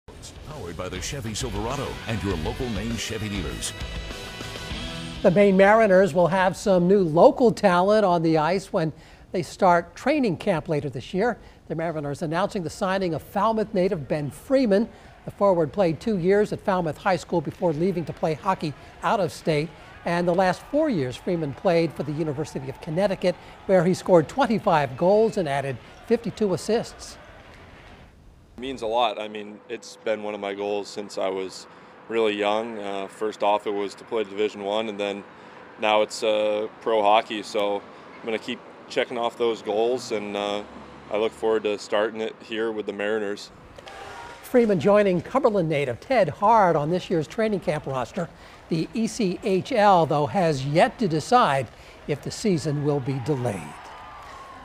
Maine Mariners sign Falmouth native
The Maine Mariners signed former Falmouth high school standout Ben Freeman.